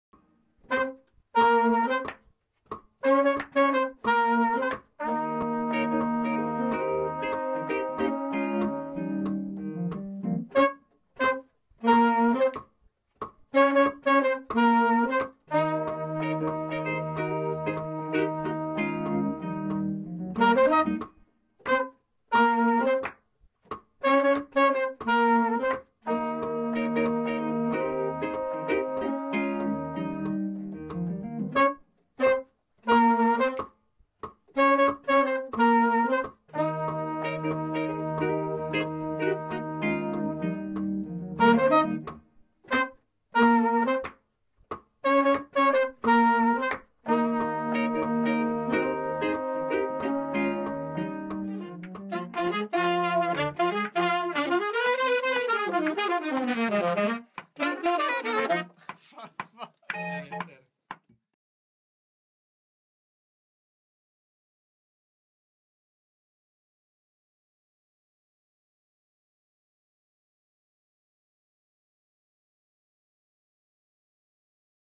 Junk koncert készülődés, fúvós szekció próba
Junk csendes ülős fúvós szekció próba. Várunk mindenkit a Kobuci kertbe augusztus 28-án!